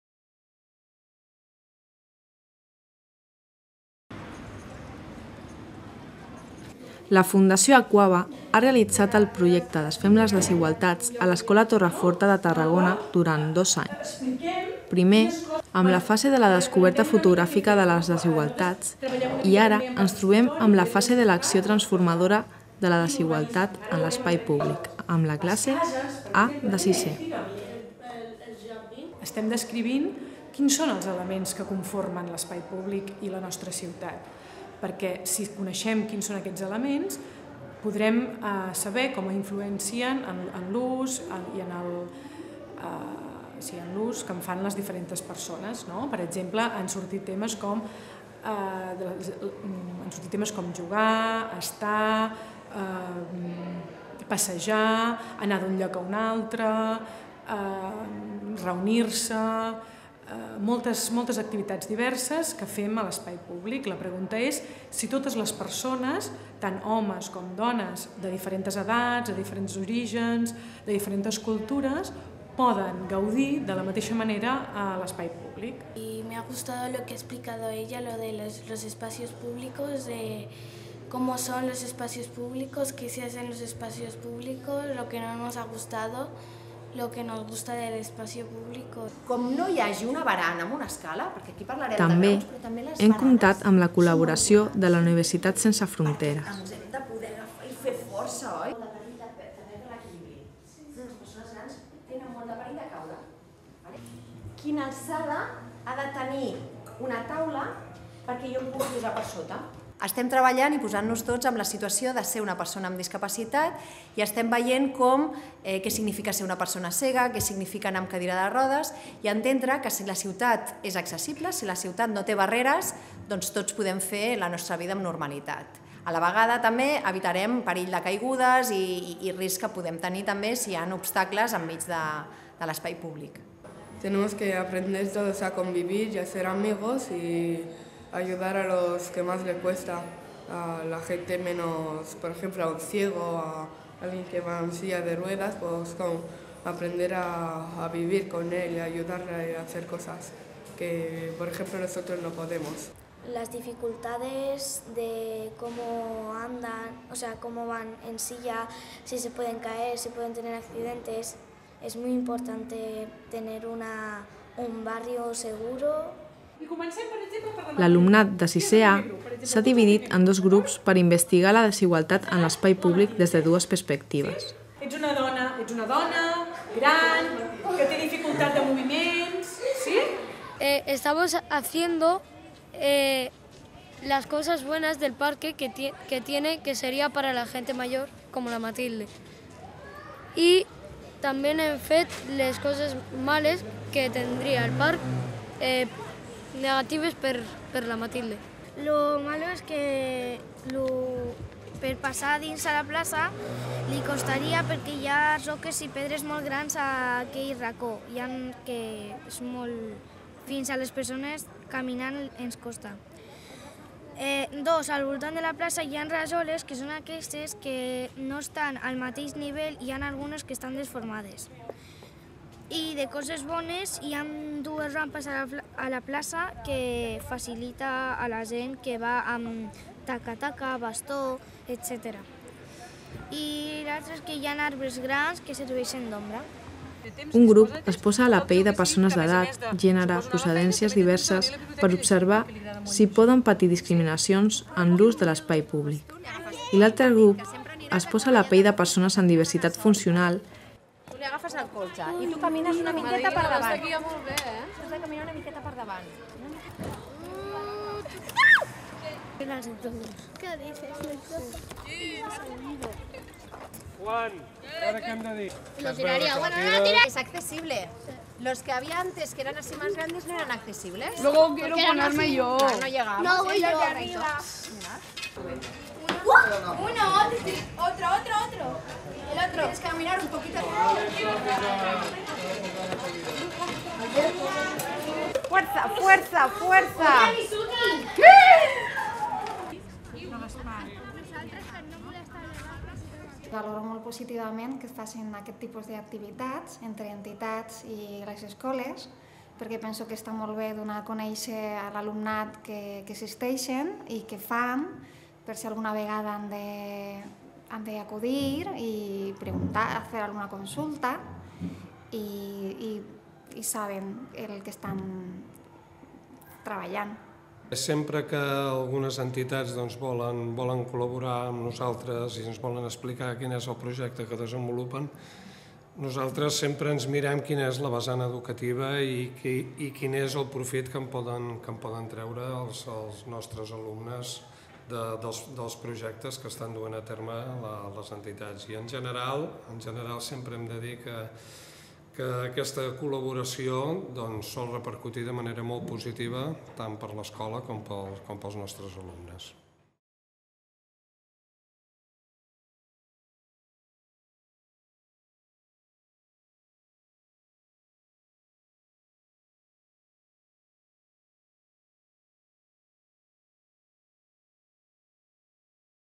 Vídeo Investiguem les discriminacions d’accés a l’entorn de l'escola Torreforta
El passat dimarts 24/10/2015, es va realitzar a l’escola Torreforta de Tarragona – classe 6è A- un taller on l’alumnat va rebre, durant la primera part de la sessió, diferents eines per part de dues entitats col·laboradores per poder analitzar l’entorn proper a l’escola.